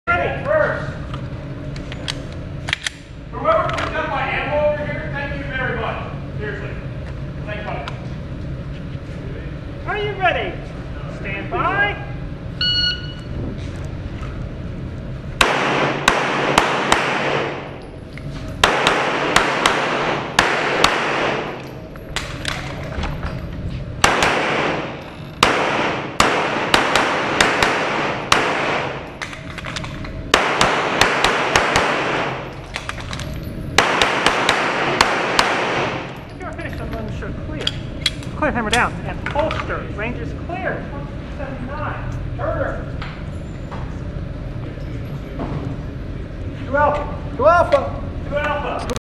GPSL Stage 3 - The Sleeper Has Awoken - Aug 23, 2016
Stage 3 at the August 23, 2016 Gwinnett Practical Shooting League match, which is held at Range, Guns & Safes in Norcross.

Another decently quick run, but low points pushed me down. Oh well!

Glock 34
Dawson sights